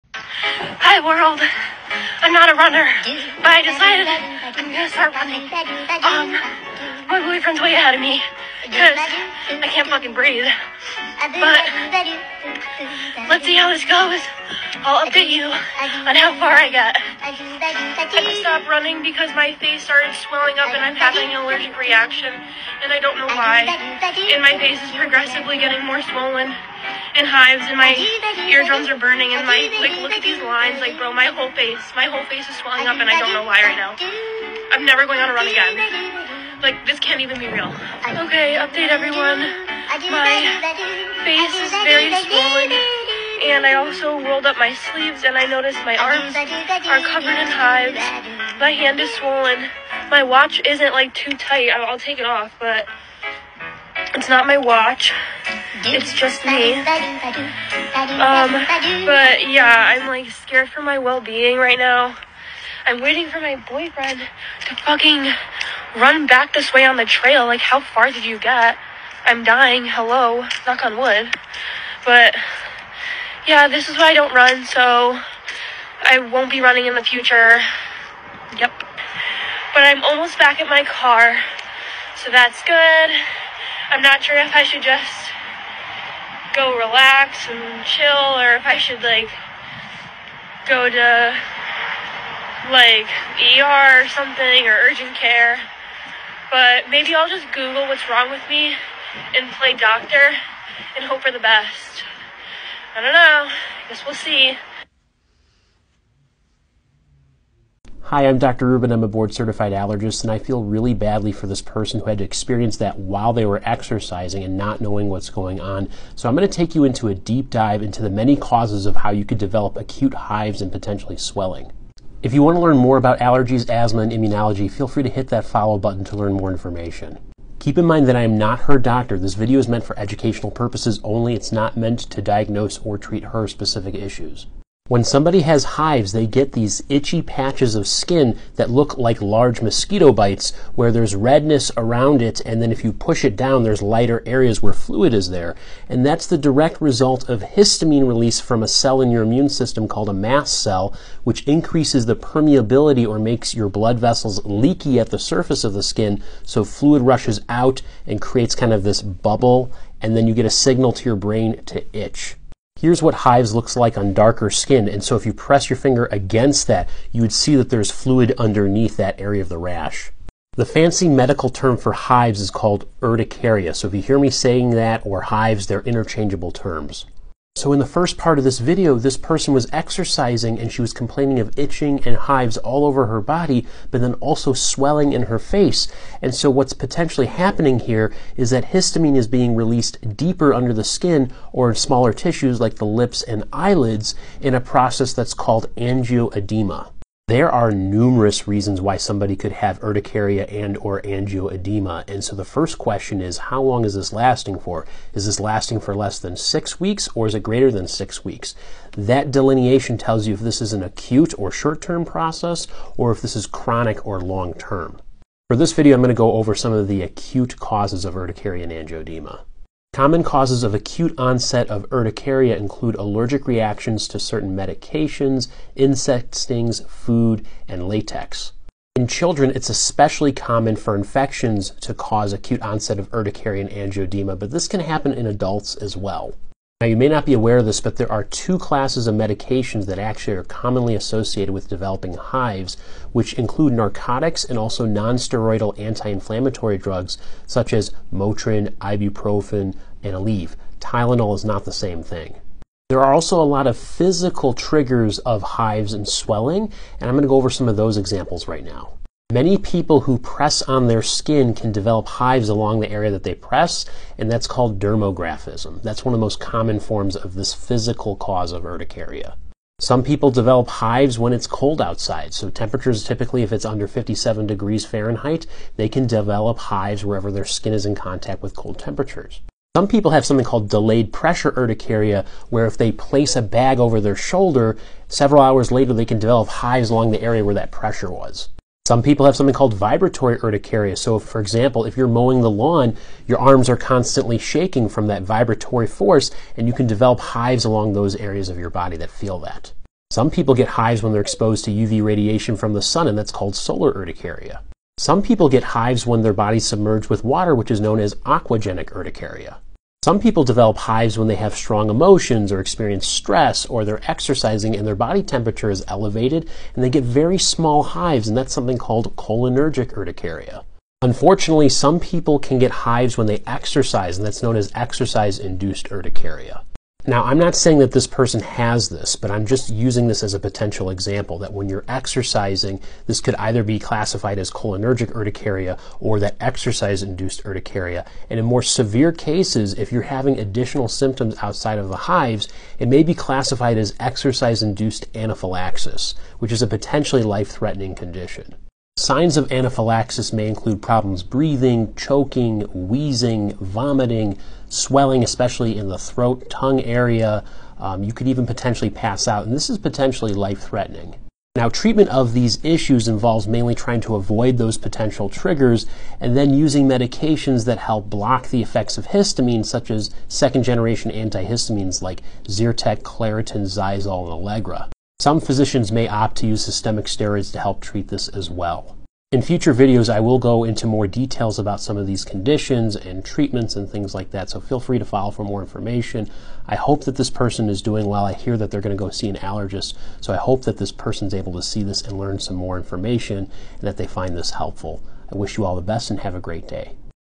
Allergic to exercise? And other causes of hives
There are many reasons why you can get hives